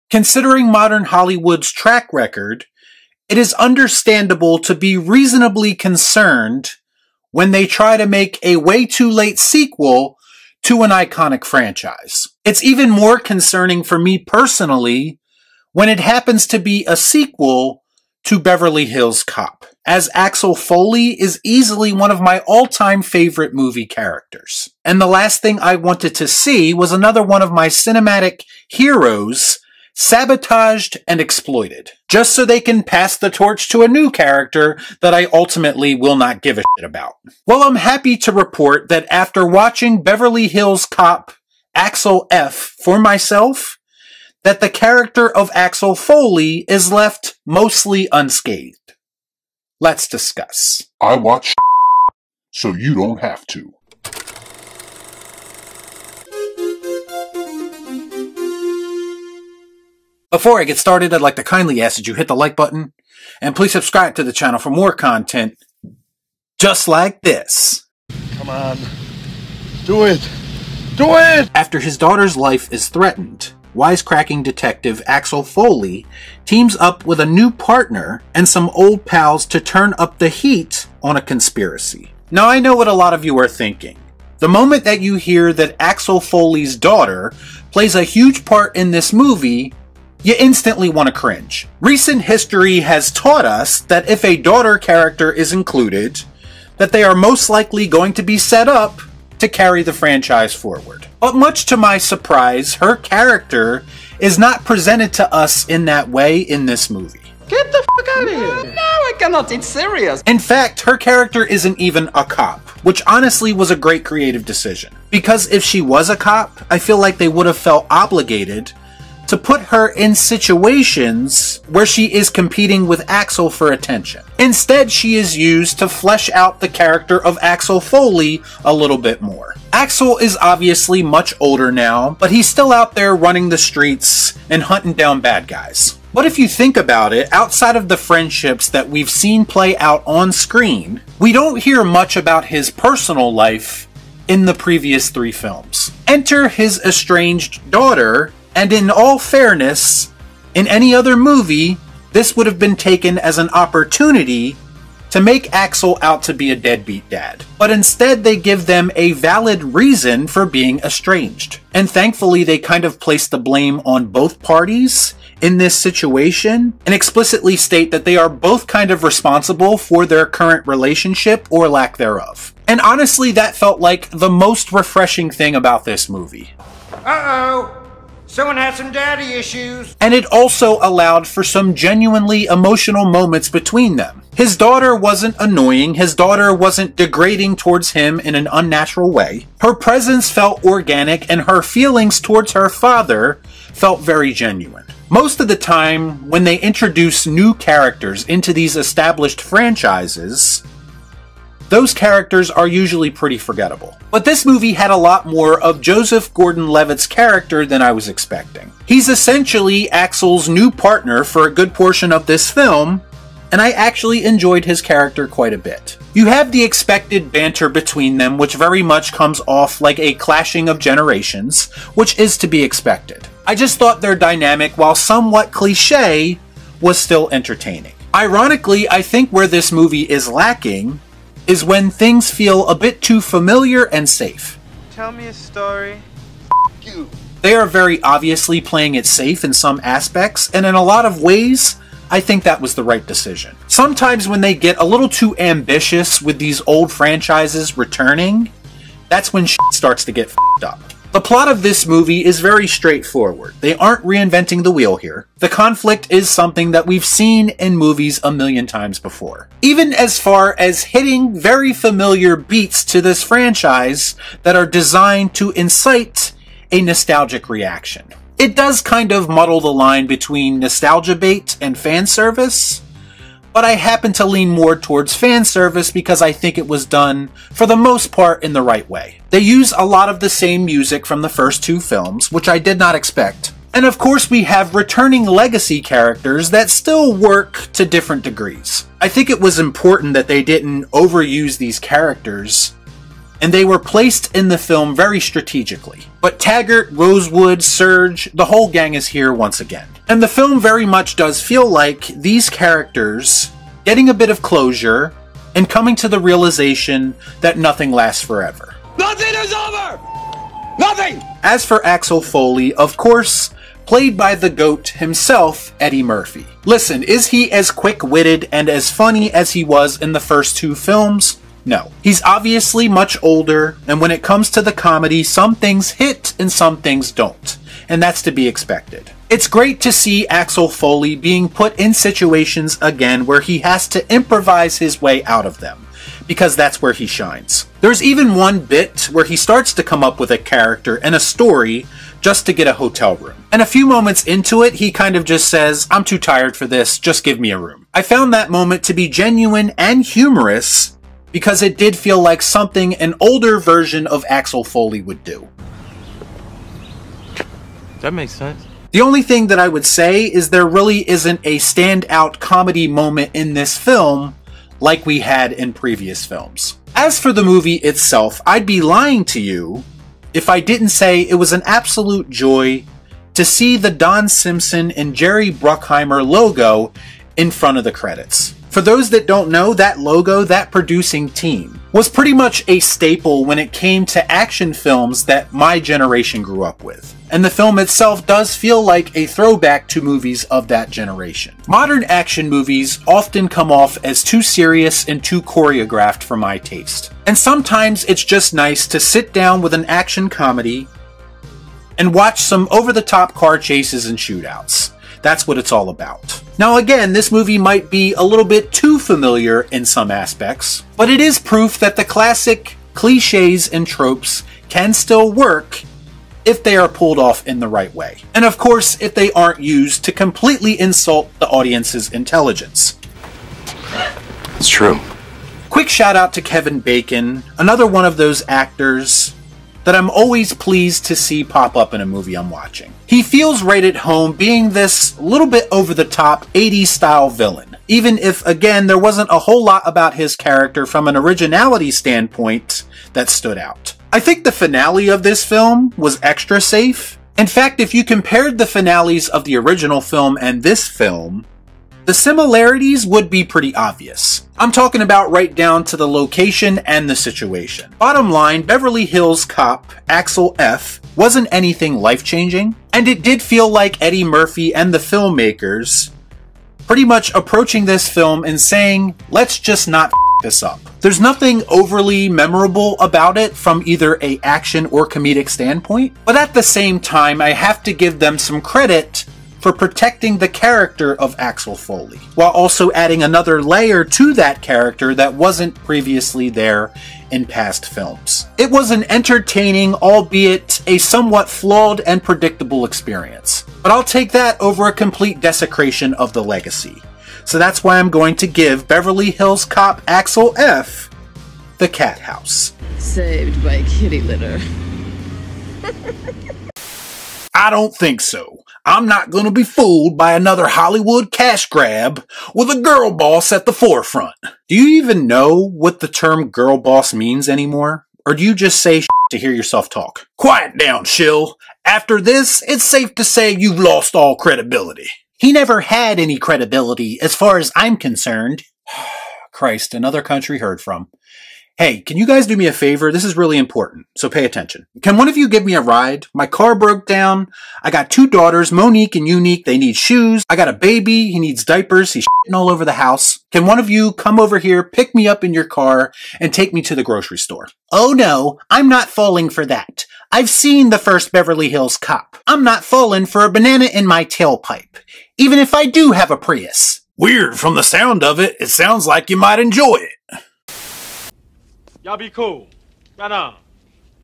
BEVERLY HILLS COP 4 (2024) IS RIDICULOUSLY SAFE! BUT DOES IT RUIN AXEL F? | REEL SHIFT
My review of Beverley Hills Cop Axel F (2024) the much too late sequel that tries to recapture the magic!

MY MOVIE RATING SYSTEM:
Masterpiece - Make Em' An Offer